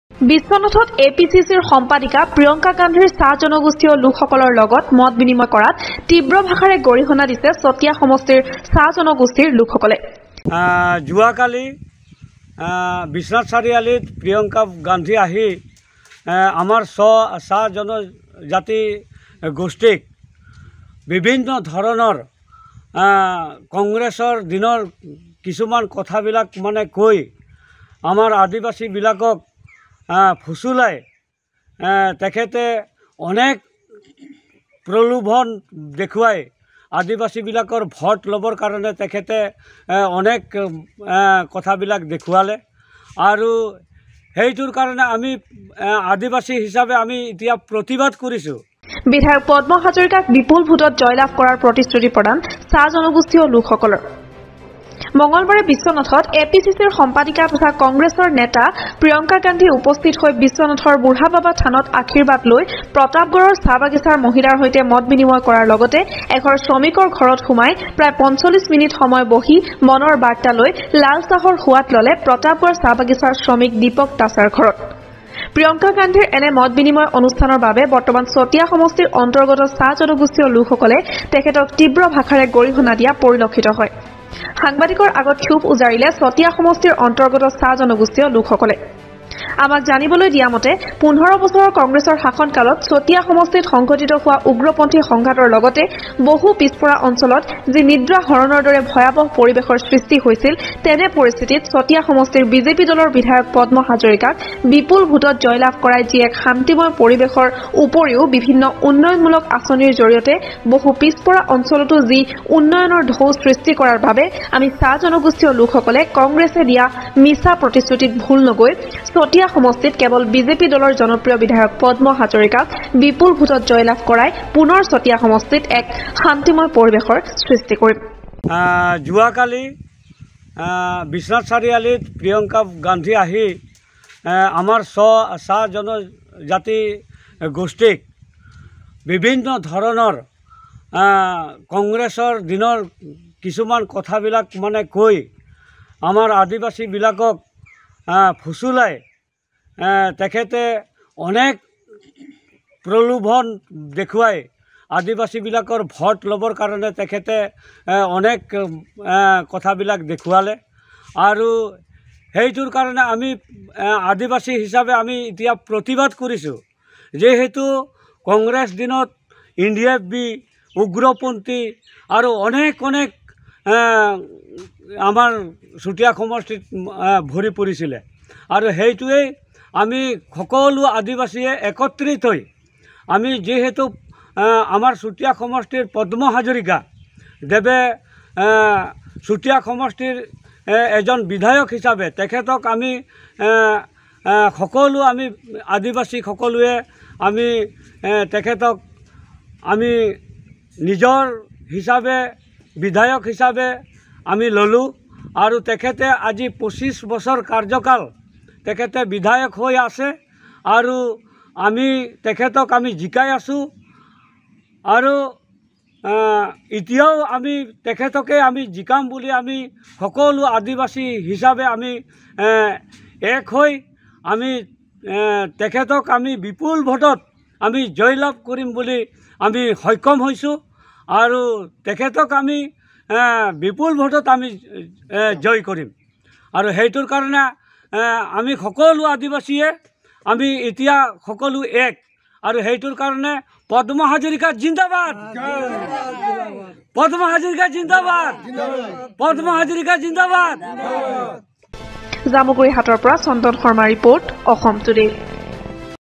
বিশ্বনাথত এপিচিচিৰ সম্পাদিকা প্ৰিয়ংকা গান্ধী
বিশ্বনাথত এপিচিচিৰ  সম্পাদিকা প্ৰিয়ংকা গান্ধীৰ চাহ জনগোষ্ঠীৰ লোকসকলৰ লগত মত-বিনিময় কৰাত তিব্ৰ ভাষাৰে গৰিহনা দিছে চতিয়া সমষ্টিৰ চাহ-জনগোষ্ঠী লোকসকলে : বিধায়ক পদ্ম হাজৰিকাক বিপুল ভোটত জয়লাভ কৰাৰ প্ৰতিশ্ৰুতি প্ৰদান চাহ জনগোষ্ঠী লোকসকলৰ ।